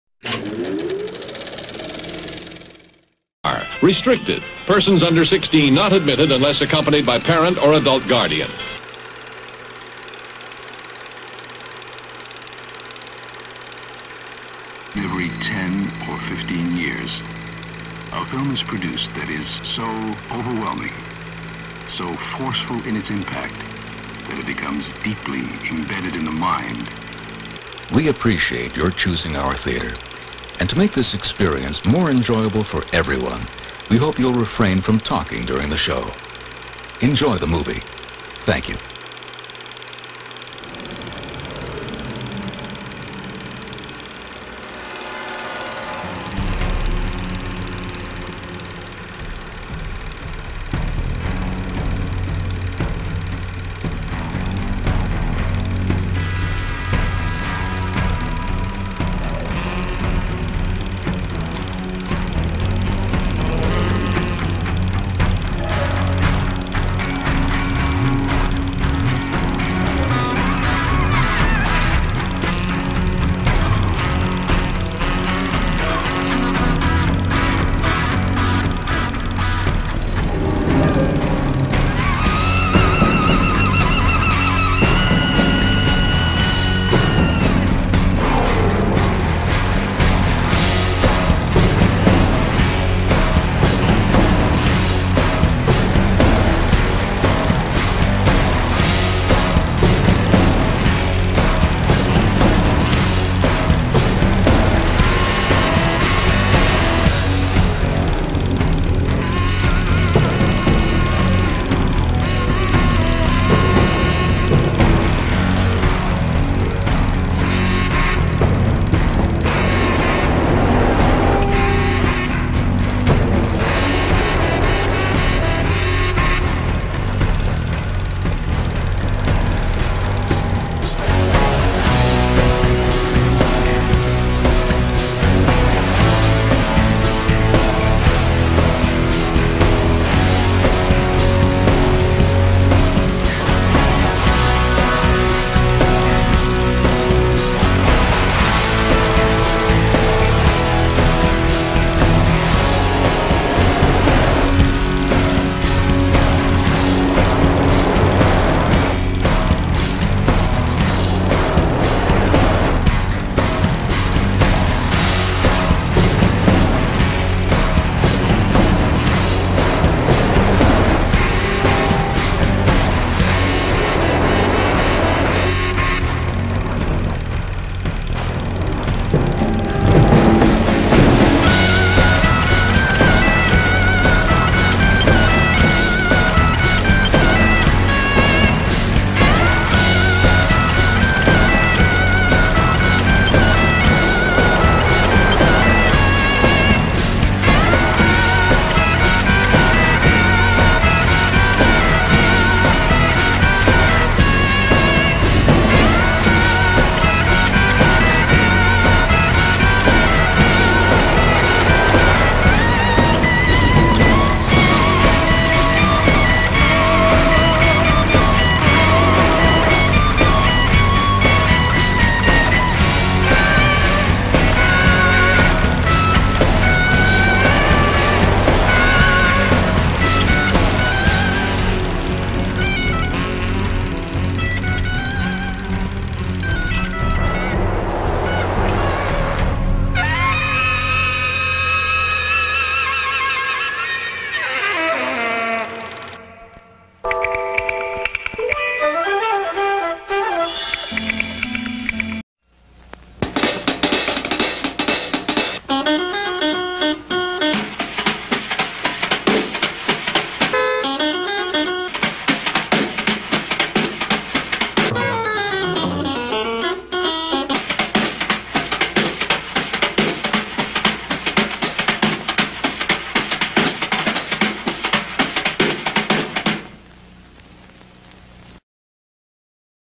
-==BLOOD AND BUBBLES SERIES==- 2016
Revised version of our wacky vintage underwater trailer, just having some fun with it .

-==SCUBA BABES.COM==-